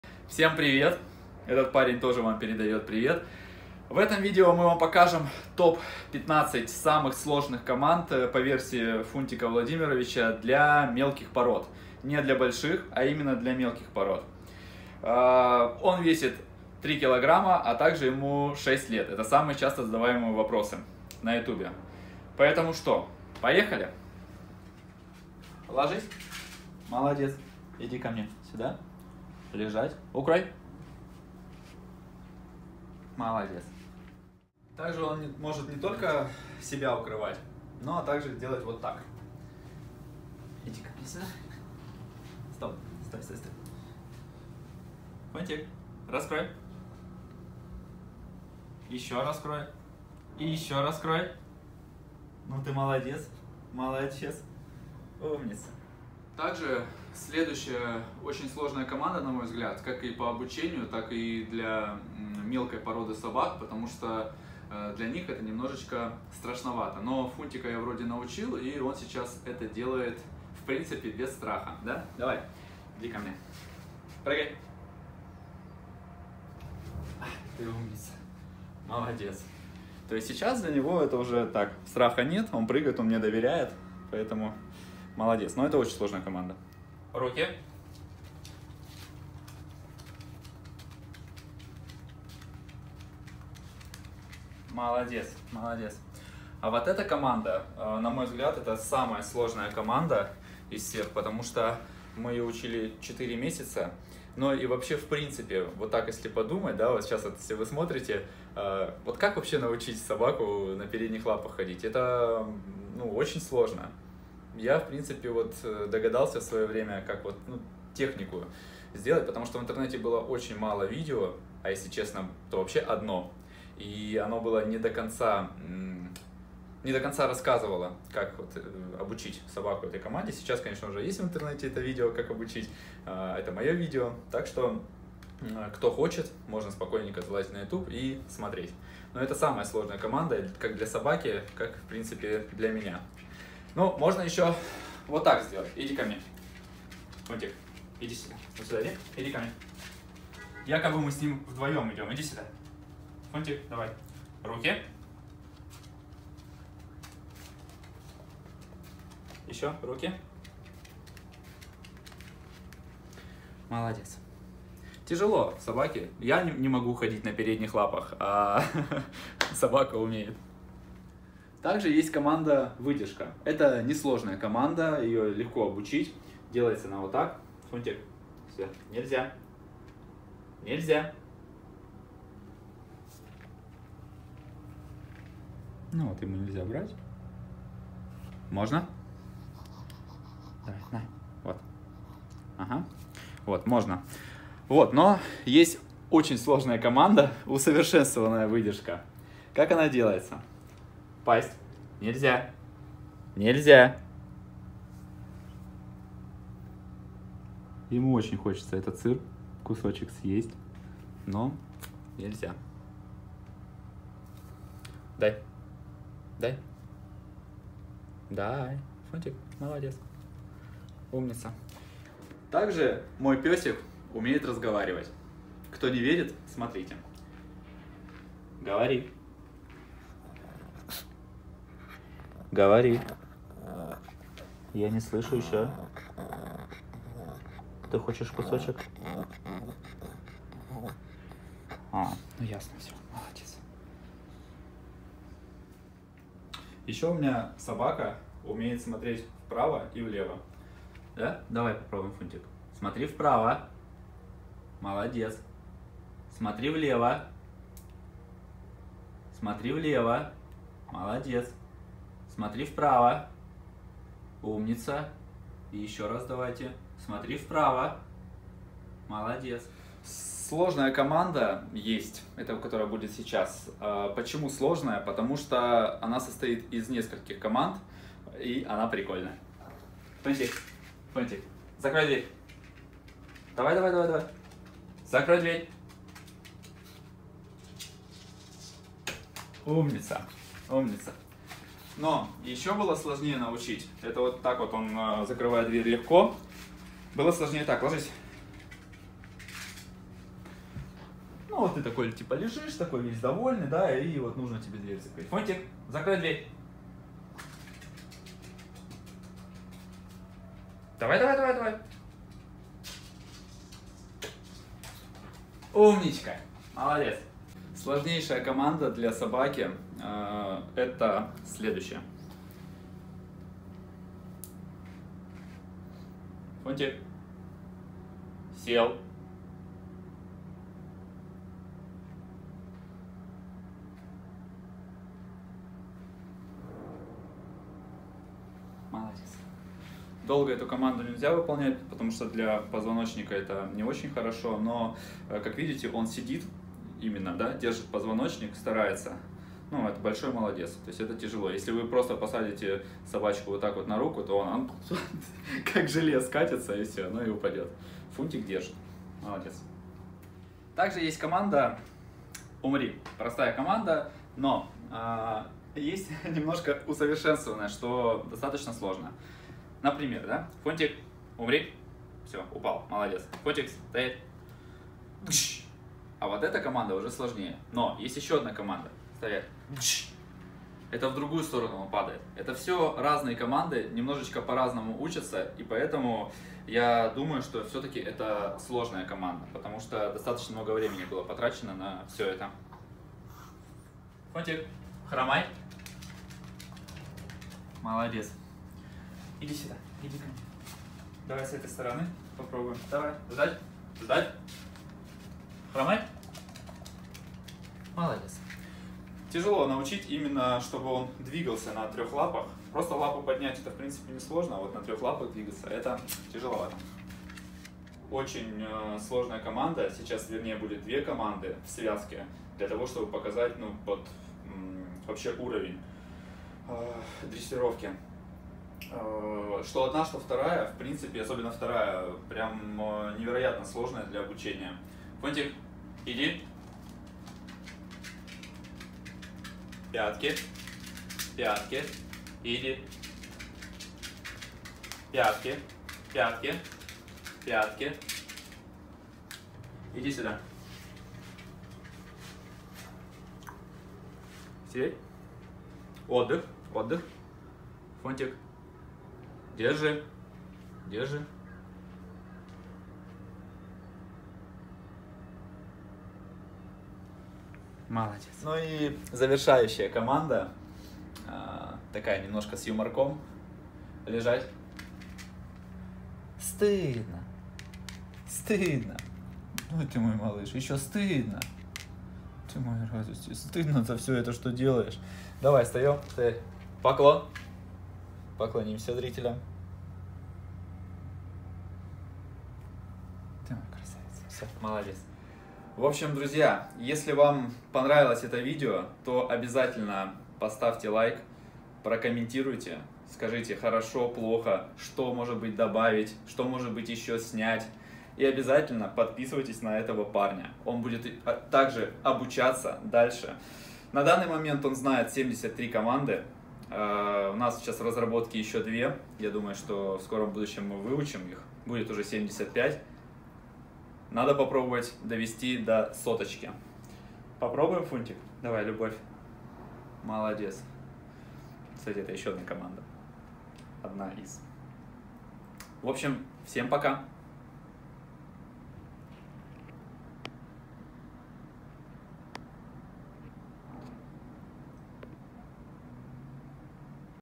Топ 15 самых сложных команд для собаки (Top 15 difficult commands for dog)
Ребята, мы выбрали 15 самых сложных для выполнения/обучения команд для собаки) Скоро выложим видео с самыми лёгкими командами)
Очень прошу, помогите распространить видео. Хочется, чтобы больше людей узнали, что и мелкие породы собак очень умненькие, несмотря на размер😎
Кстати, я замечаю, что у людей начинает меняться мнение о мелких пёсиках, что не может не радовать☺️